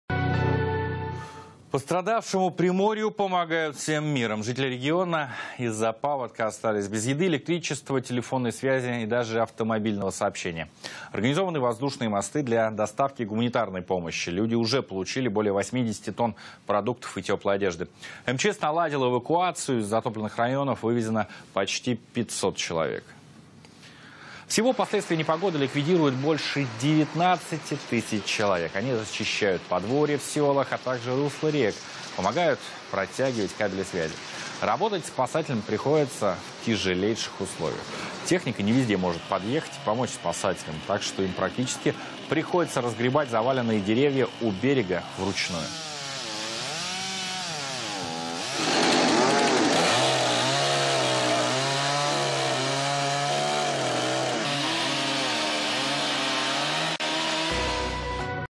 Пострадавшему от паводка Приморью помогает вся Россия
Населению Приморья доставлена гуманитарная помощь